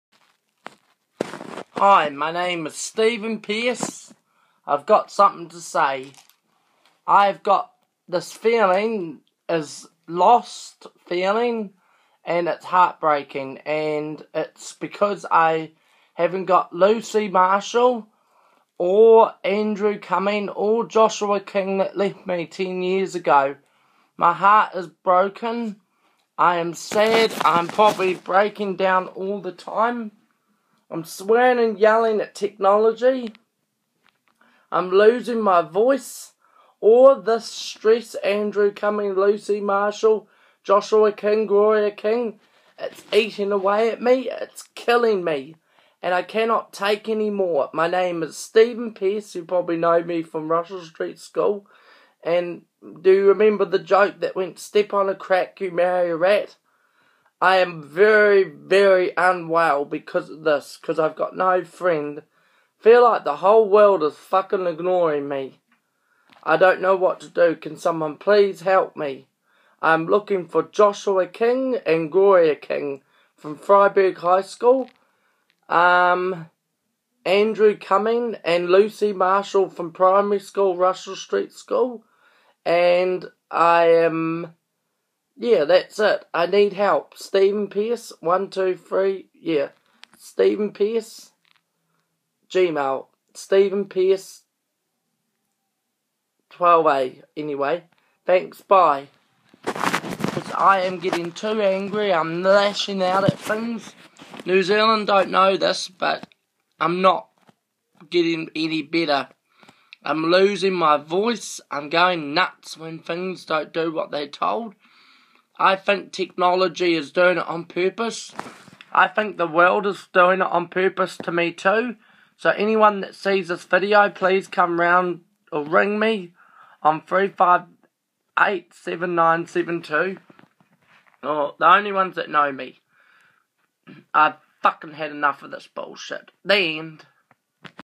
Stevenpearce looking for my friends Andrew cumming Lucy Josha king
Need help really bad because I am really lost with out Andrew cumming and lucy